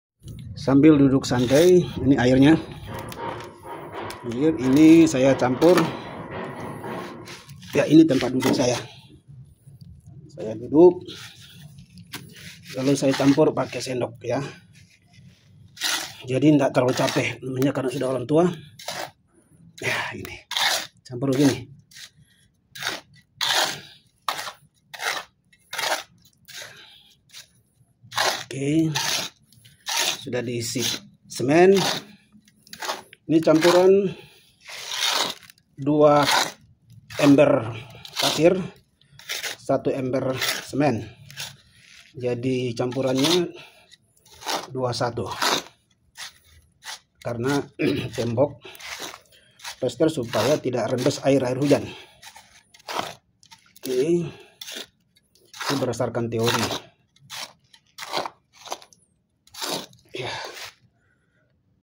tukang ini santai amat buat campuran?
Tukang di rumah sendiri jadi buat camloyran juga santai